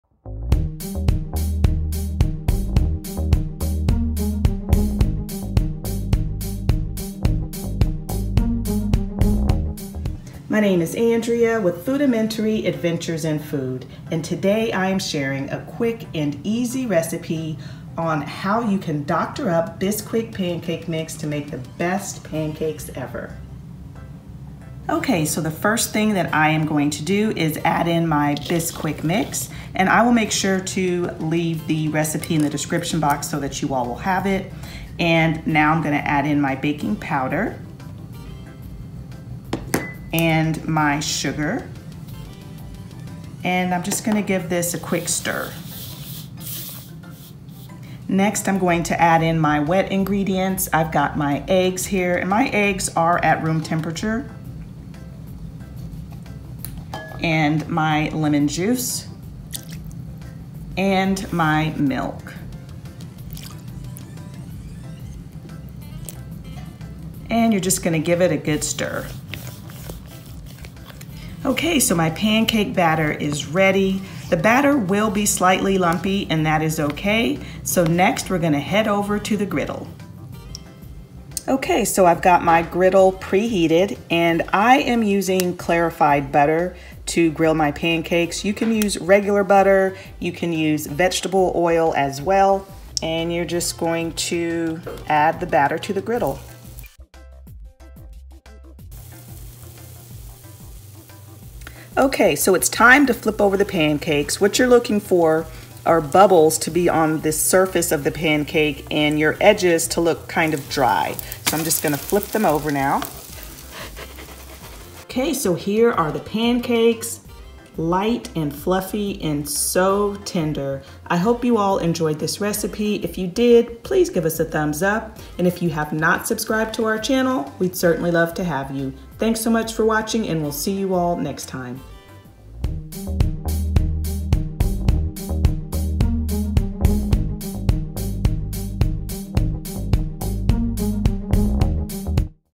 Melt In Your Mouth Pancakes | Doctored Up Bisquick Pancake Mix!
Pancake Recipe (makes 6-7 medium pancakes):
2 cups of Bisquick Pancake Mix
1 cup of milk, room temp
1 tablespoon of sugar
2 tablespoons of lemon juice
2 teaspoons of baking powder
2 eggs, room temp

Stir until well blended.  Pour pancake batter on hot griddle until golden brown.